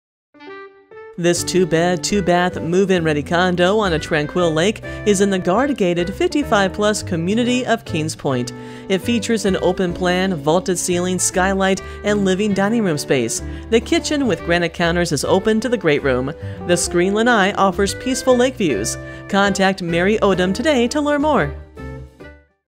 2519 New Haven Circle Sun City Center, FL | ColdwellBankerHomes.com
2519 New Haven Circle, Sun City Center, FL

Kings Point, two bedroom, two bathroom condominium on a tranquil lake that has a screened lanai and a garage. Enjoy the best of 55+ living with this move-in, ready unit. Open floor plan has vaulted ceiling, skylight for natural light, living room and dining room combination. Washer and dryer include